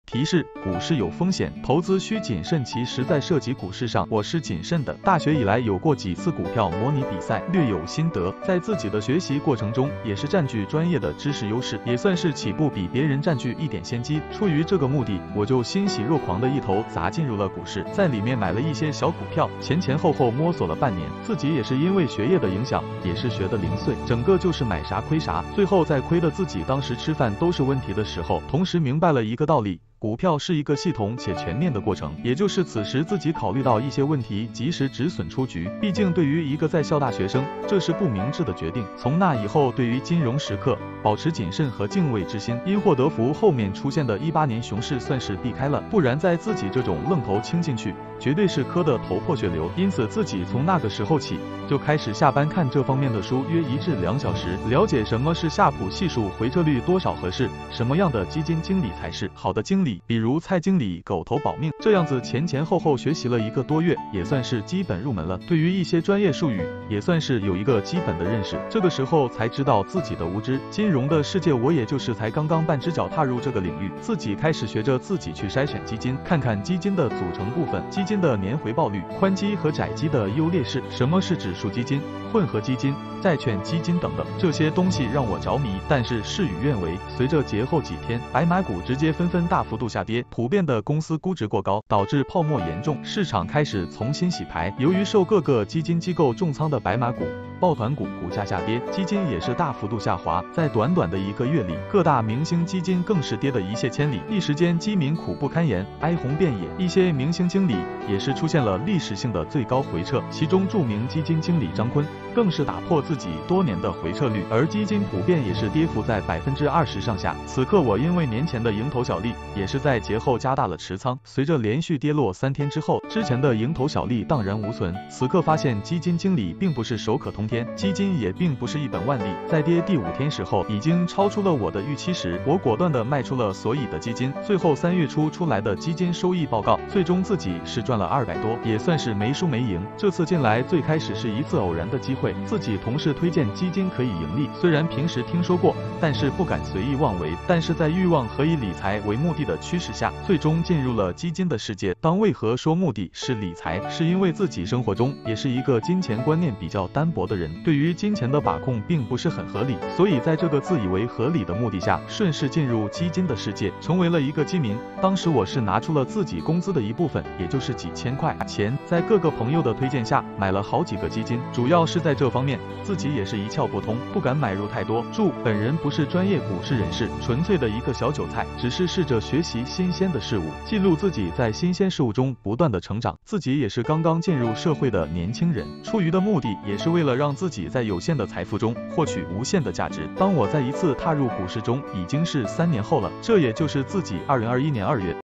前言 || 初入股市風雲，韭菜的自我修養
提示：股市有風險，投資需謹慎其實在涉及股市上，我是謹慎的，大學以來有過幾次股票模擬比賽，略有心得，在自己的學習過程中也是占據專業的知識優勢，也算是起步比別人占據一點先機，出於這個目的我就欣喜若狂的一頭砸進入了股市，在裡面買了一些小股票，前前後後摸索了半年，自己也是因爲學業的影響也是學得零碎，整個就是